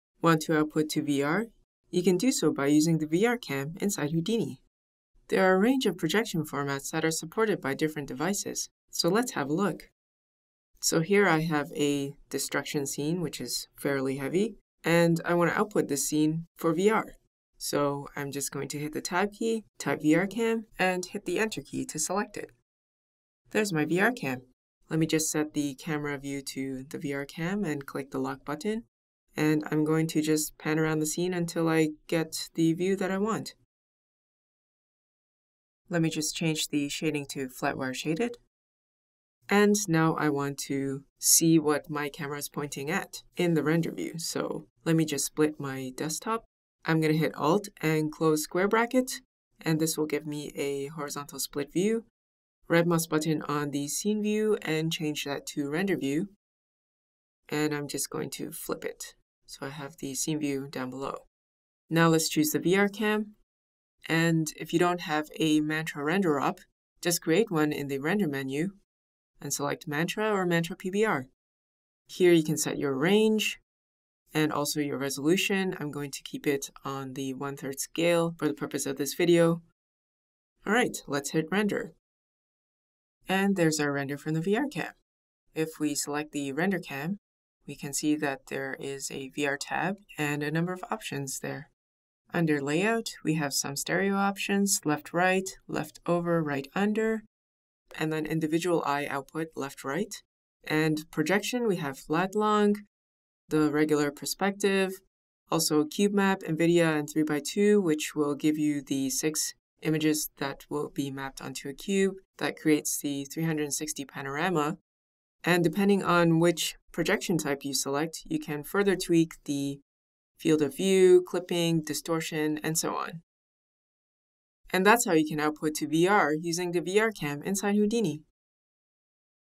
VR Lens Camera
The VR Camera allows you to output your scene to several formats which are supported by various devices. You can set your renders to Projection types Latlong, Perspective, Cube Map – NVIDIA and Cube Map – 3x2 and Layout you can choose Left/Right, Left Over/Right Under, Left and Right. Additionally depending on the projection type, there are controls for Eye Separation, Eye to Neck Distance, Field of View, Near/Far Clipping and more.